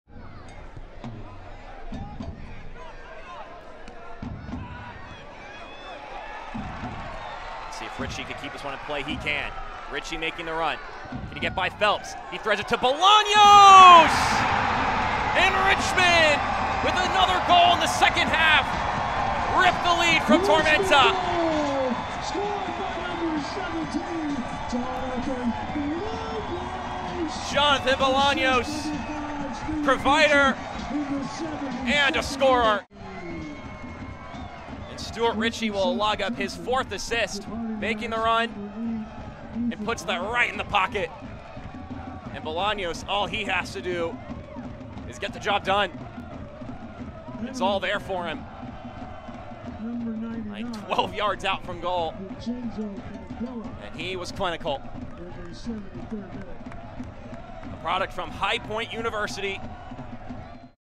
Jonathan Bolanos with a Goal vs. South Georgia Tormenta FC
Jonathan Bolanos (Richmond Kickers) with a Goal vs. South Georgia Tormenta FC, 09/07/2022